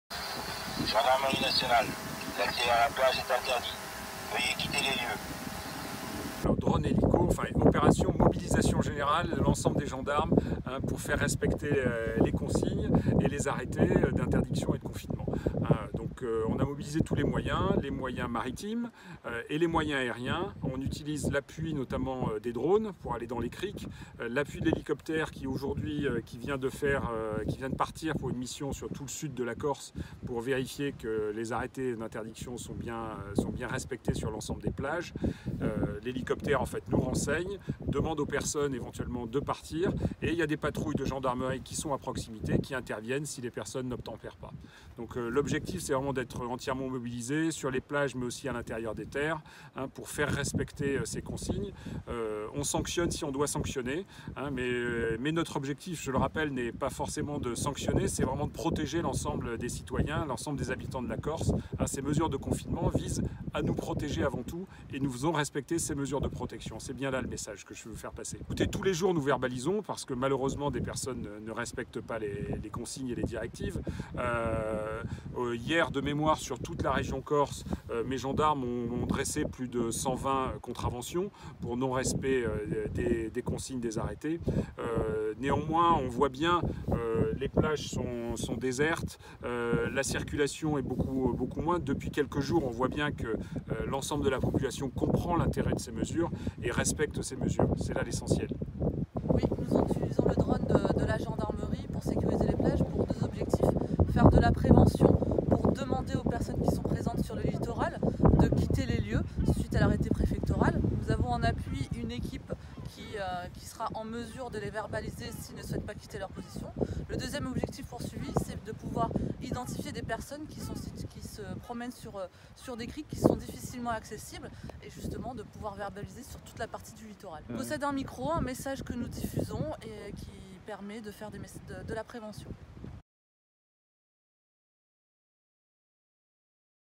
Confinement : les gendarmes avec Hélico et Drone survolent les plages de Corse
On ne badine avec les règles de confinement imposées dans le cadre de la pandémie du Covid-19. Ce samedi à 14h30 la gendarmerie de Corse a procédé sur la plage de Porticcio à une opération spectaculaire pour inciter la population corse à demeurer chez elle en cette première journée de week-end. Drone et hélicoptère ont, en effet utilisé, par les gendarmes pour à partir d'un haut-parleur fixé sur un drone rappeler que l'accès aux plages était interdit et qu'il fallait quitter les lieux. Sur place le général Tony Mouchet, commandant la légion de gendarmerie de Corse et le chef d'escadron Sabrina Perrault ont expliqué comment à partir de l'observation faite par le drone, les gendarmes à bord de l'hélicoptère ont pu prévenir voire même verbaliser. Plus que jamais restez chez vous !